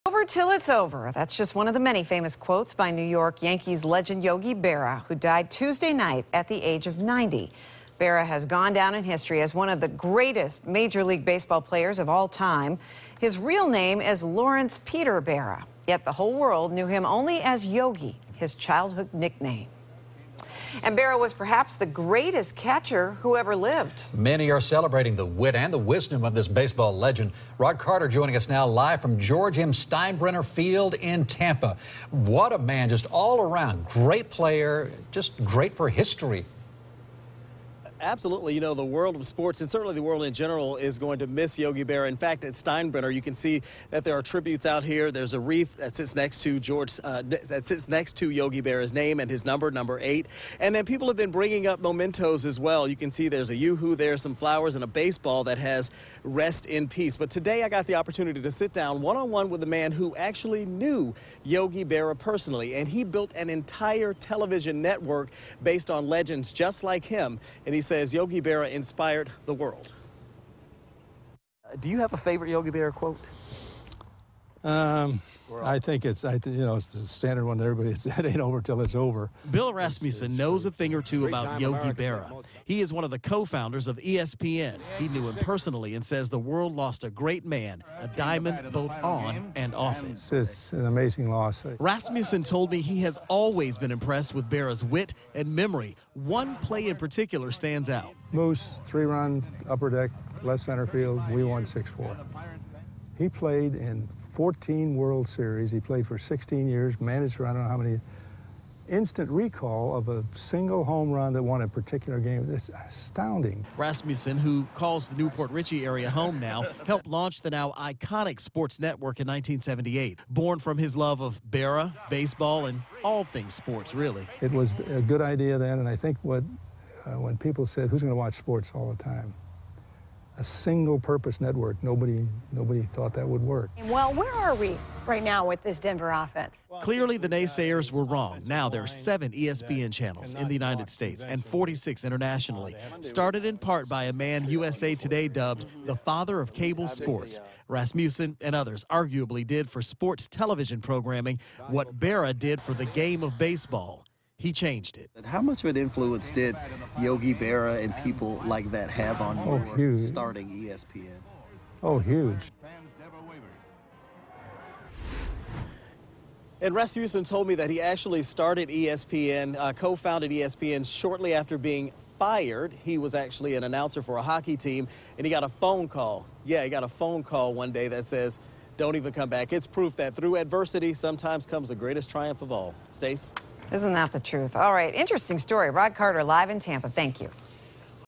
Co-founder of ESPN remembers Yankee’s legend Yogi Berra
Yogi Berra died Tuesday from natural causes.